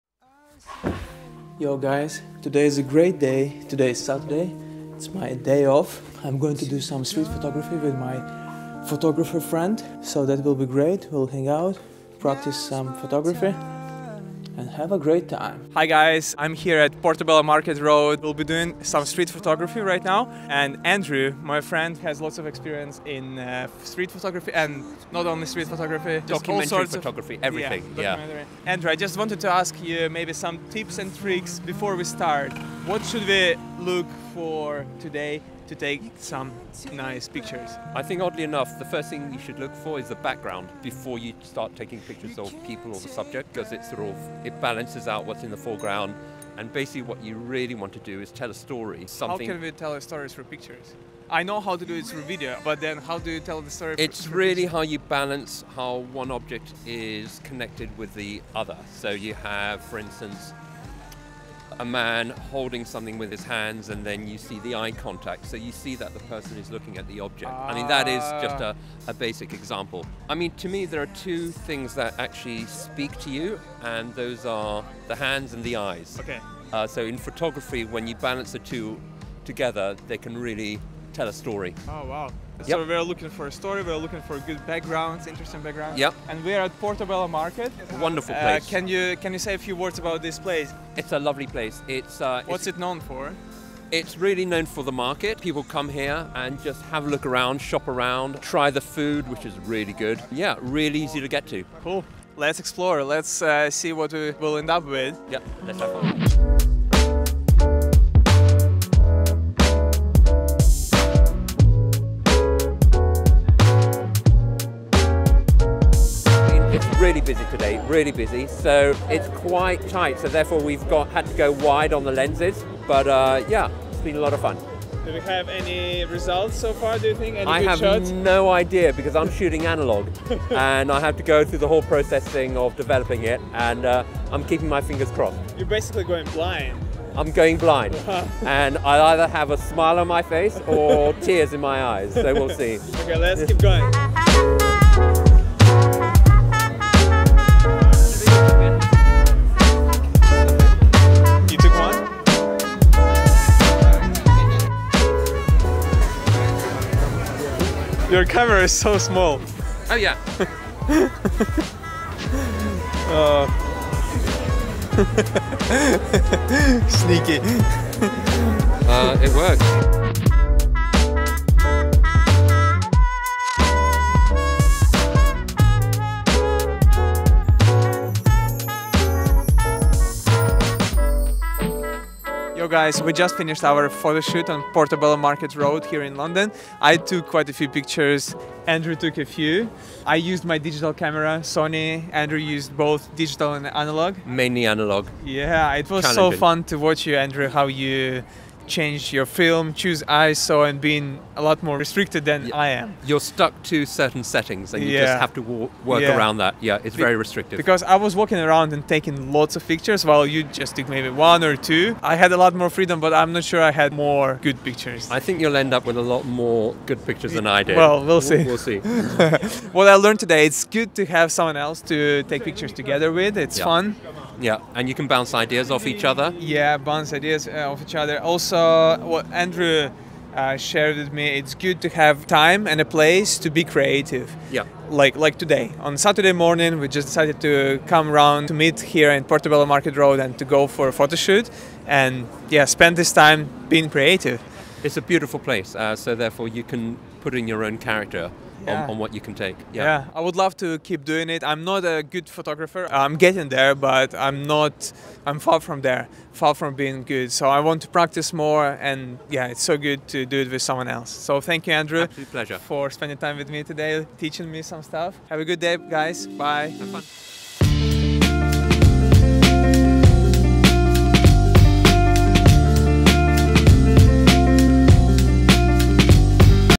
Street Photography in London | Portobello Road Market
Spending a few fun hours of street photography in London with a photographer friend Andrew at Portobello Road Market.